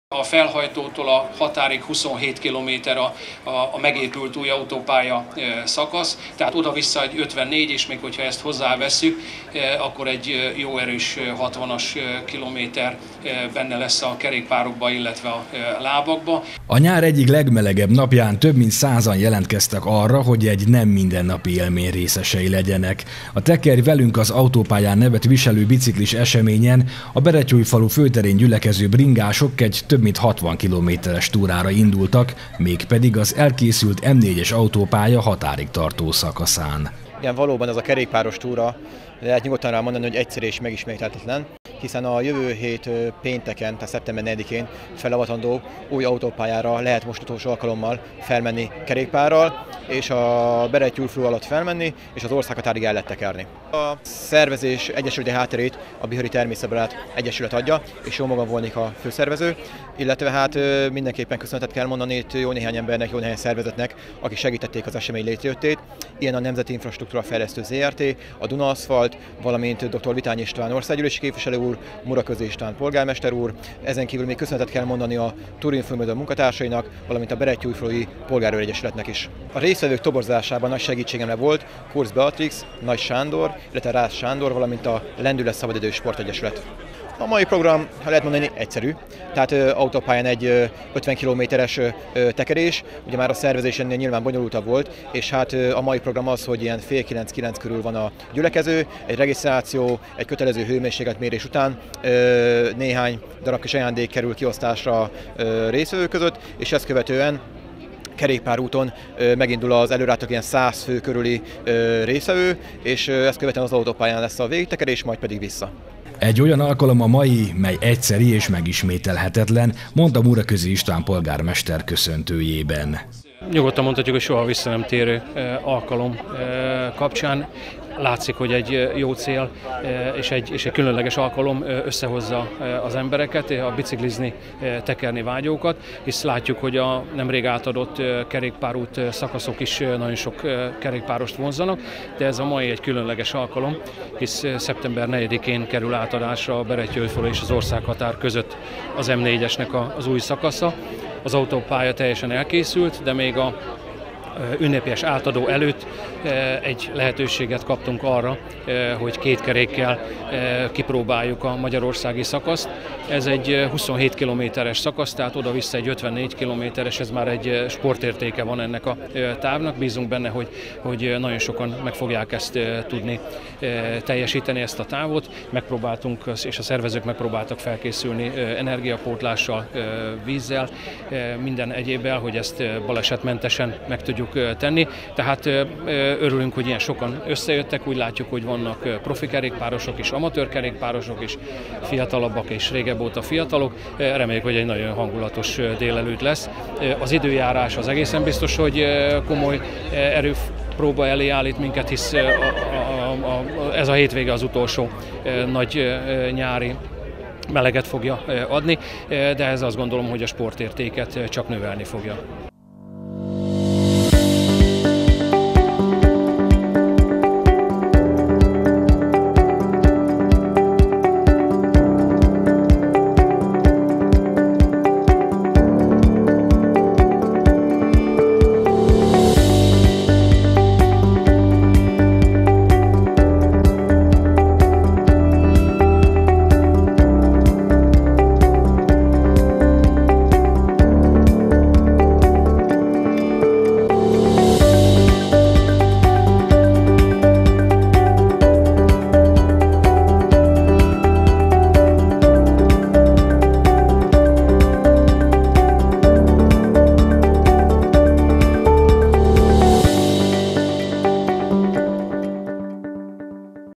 Biciklivel már felavatták az M4-es autópálya határig tartó szakaszát
A nyár egyik legmelegebb napján több mint százan jelentkeztek arra, hogy egy nem minden napi élmény részesei legyenek. A „Tekerj velünk az autópályán” nevet viselő biciklis eseményen, a Berettyóújfalu főterén gyülekező bringások egy több mint 60 kilométeres túrára indultak, mégpedig az elkészült M4-es autópálya határig tartó szakaszán.